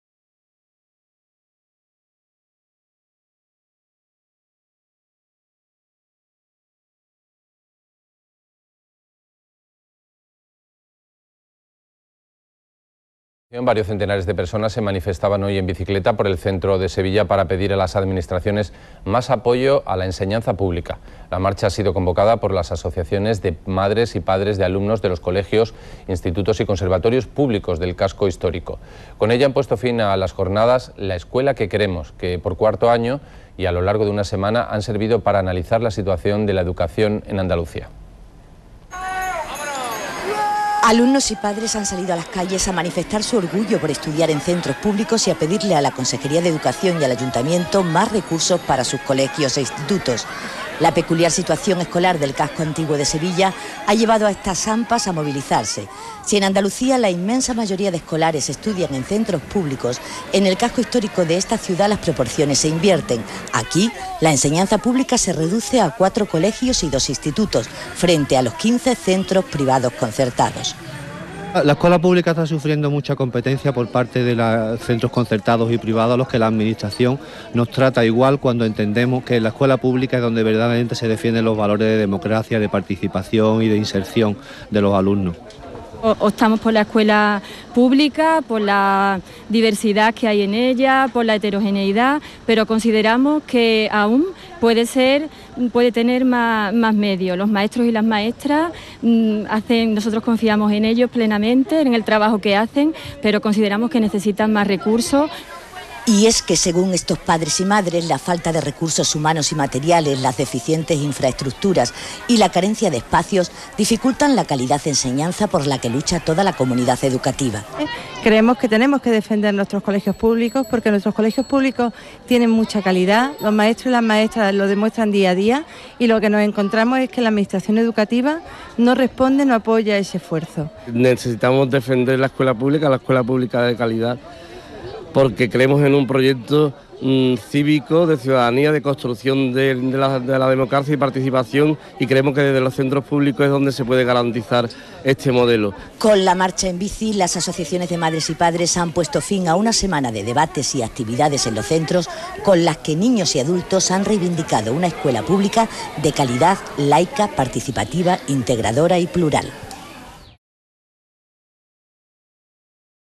La Escuela que queremos 2008
Las Ampas del centro de Sevilla han culminado el IV Encuentro "La escuela que queremos" con una manifestación en bici para reivindicar la construcción del nuevo colegio público en El Valle.